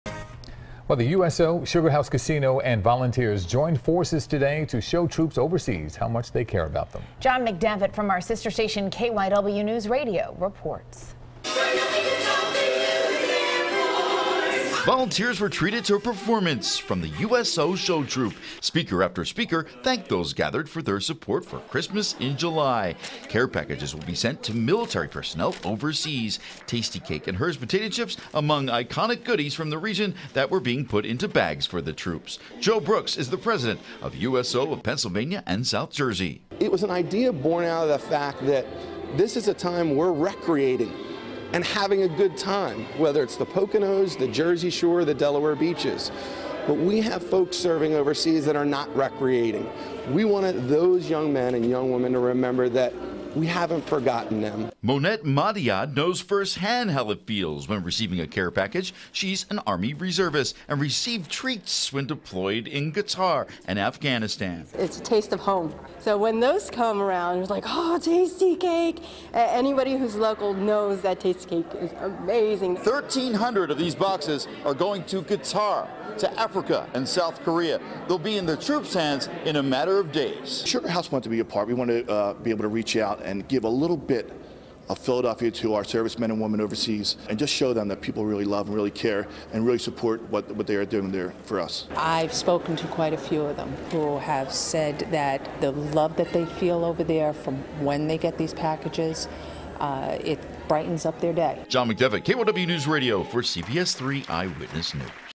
USO Sends Care Packages To Troops Overseas
John McDevitt reports.